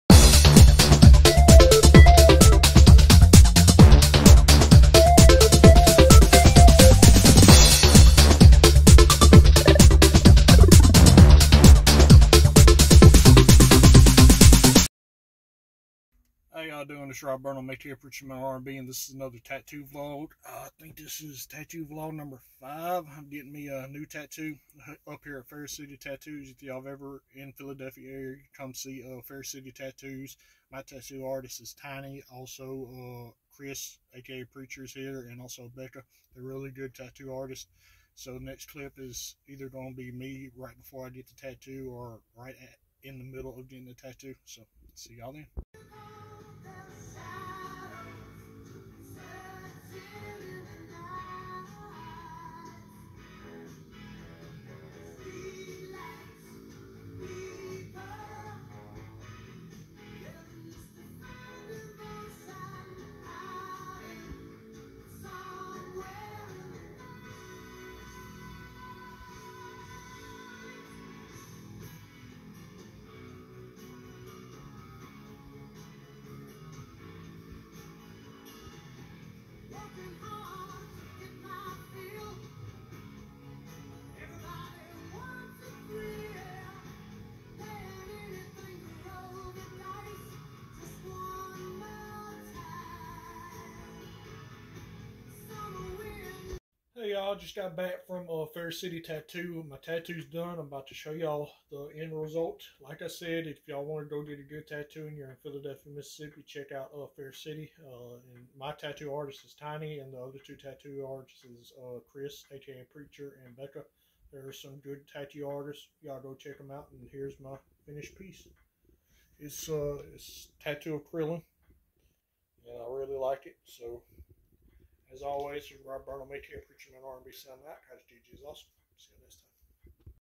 Tattoo vlog #5.
This is another tattoo vlog and this time i got a tattoo of krillin.